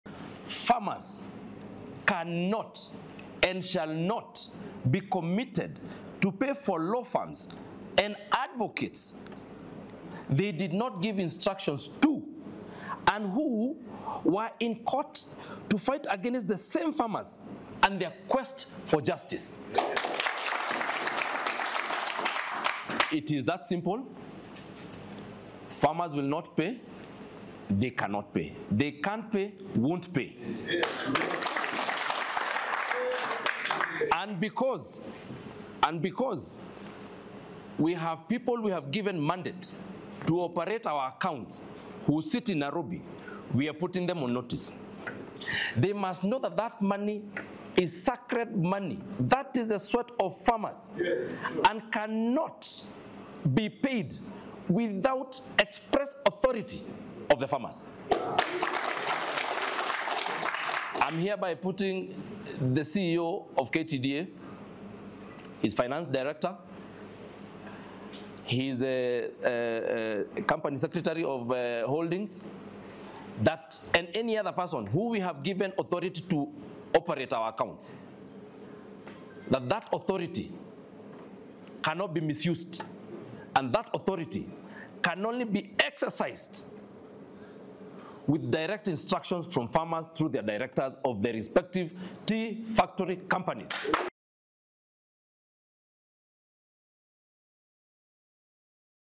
Directors of tea factories in Mt. Kenya say there has been a resurgence of cartels
Directors of smallholder tea factory companies from the Mount Kenya region have raised alarm over the resurgence of cartels in the tea sector.